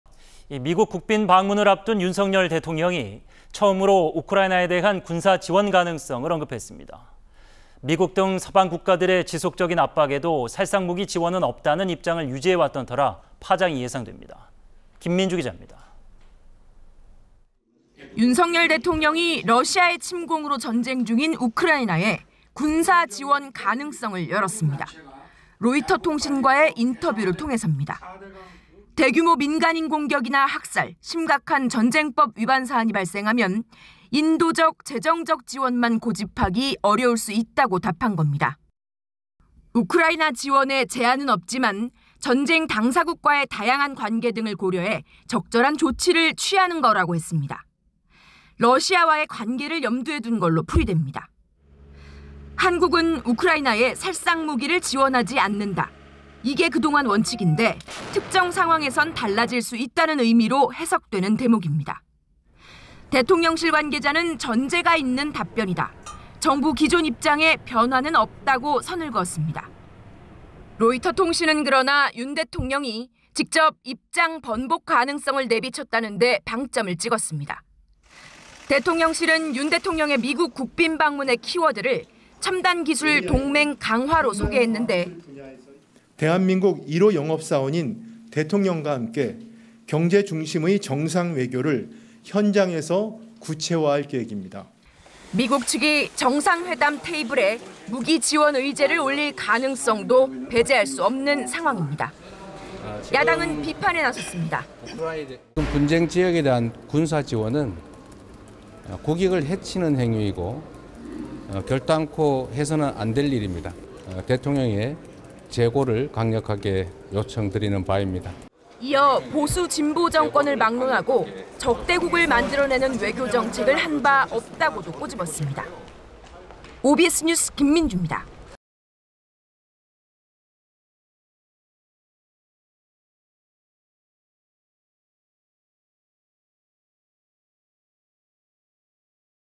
尹 '우크라 군사 지원' 시사.. 한미 회담 겨냥한 협상 카드? / OBS 뉴스O
【앵커】

미국 국빈 방문을 앞둔 윤석열 대통령이 처음으로 우크라이나에 대한 군사 지원 가능성을 언급했습니다.

미국 등 서방 국가들의 지속적인 압박에도 살상 무기 지원은 없다는 입장을 유지해왔던 터라 파장이 예상됩니다.

김민주 기자입니다.

【기자】

윤석열 대통령이 러시아의 침공으로 전쟁 중인 우크라이나에 군사 지원 가능성을 열었습니다.

로이터통신과의 인터뷰를 통해섭니다.

"대규모 민간인 공격이나 학살, 심각한 전쟁법 위반 사안이 발생하면 인도적, 재정적 지원만 고집하기 어려울 수 있다"고 답한 겁니다.

우크라이나 지원에 제한은 없지만, 전쟁 당사국과의 다양한 관계 등을 고려해 적절한 조치를 취하고 있는 거라고 했습니다.

러시아와의 관계를 염두에 둔 걸로 풀이됩니다.

한국은 우크라이나에 살상 무기를 지원하지 않는다, 이게 그동안 원칙인데, 특정 상황에선 달라질 수 있다는 의미로 해석되는 대목입니다.

대통령실 관계자는 전제가 있는 답변이다, 정부 기존 입장에 변화는 없다고 선을 그었습니다.

로이터통신은 그러나 윤 대통령이 직접 입장 번복 가능성을 내비쳤다는데 방점을 찍었습니다.

대통령실은 윤 대통령의 미국 국빈 방문의 키워드를 첨단 기술 동맹 강화로 소개했는데,

[최상목 / 대통령실 경제수석: 대한민국 1호 영업사원인 대통령과 함께 경제 중심의 정상 외교를 현장에서 구체화할 계획입니다.]

미국 측이 정상회담 테이블에 무기 지원 의제를 올릴 가능성도 배제할 수 없는 상황입니다.

야당은 비판에 나섰습니다.

[이재명 / 더불어민주당 대표: 분쟁 지역에 대한 군사 지원은 국익을 해치는 행위이고 결단코 해서는 안 될 일입니다. 대통령의 재고를 강력하게 요청드리는 바입니다.]

보수, 진보정권을 막론하고 적대국을 만들어내는 외교정책을 한 바 없다고도 꼬집었습니다.

OBS뉴스 김민주입니다.